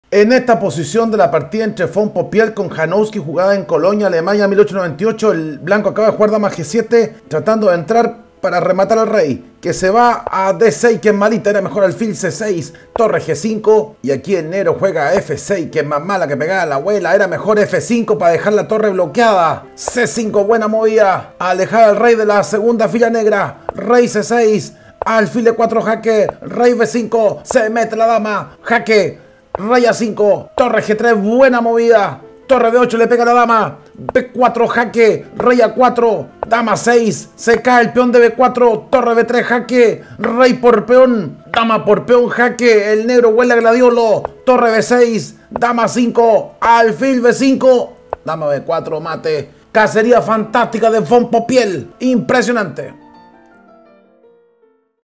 Persecución IMPLACABLE de von Popiel que termina con mate FEROZ: von Popiel - Janowski, 1898
En esta posición de la partida entre von popiel vs janowski, jugada en 1898, vemos una persecución implacable de von popiel que termina con mate feroz. Disfruta!